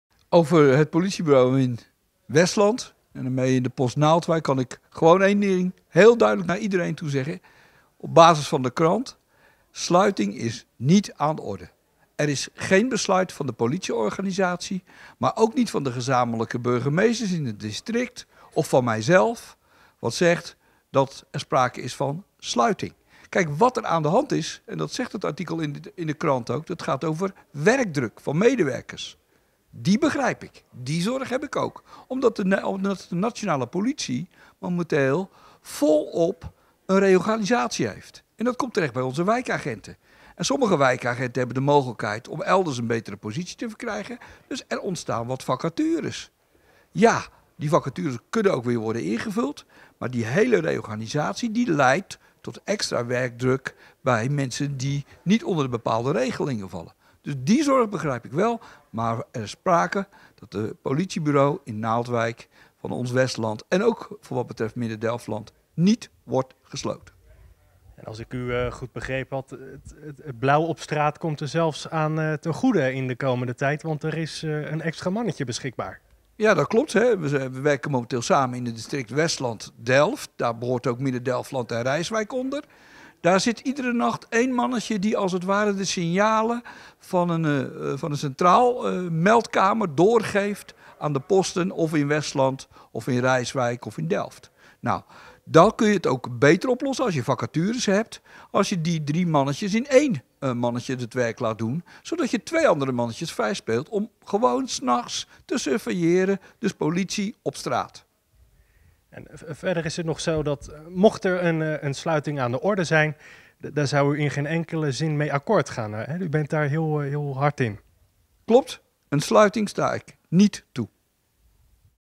'Politiebureau Naaldwijk blijft open'
Burgemeester Van der Tak is helder: het politiebureau in Naaldwijk gaat niet dicht. Wel onderschrijft de burgemeester de hoge werkdruk op het bureau.